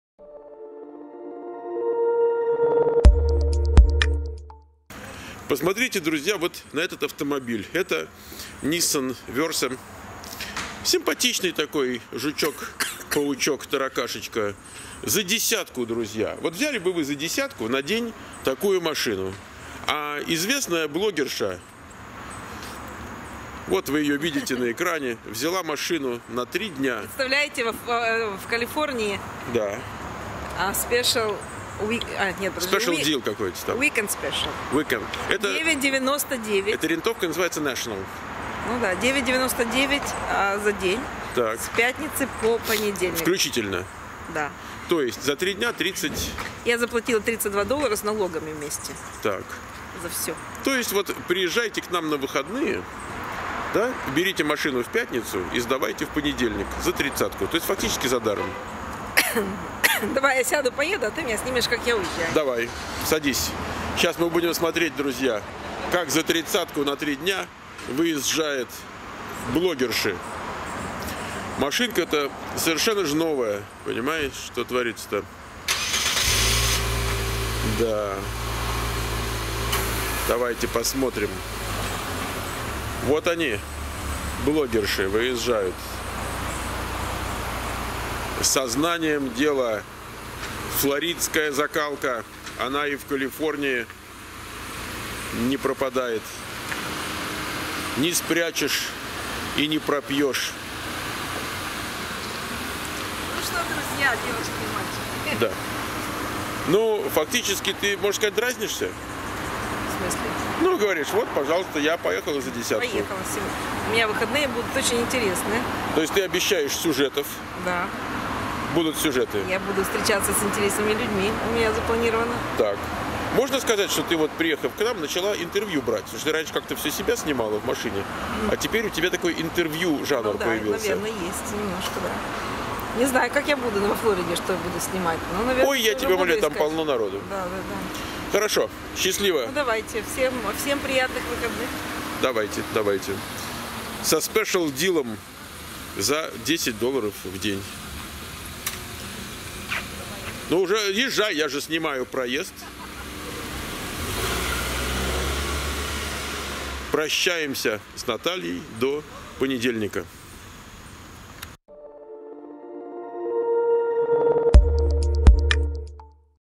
США 2663: Как Наталья Квик машину в рент брала на выходные фактически задаром
Как Наталья Квик машину в рент брала на выходные фактически задаром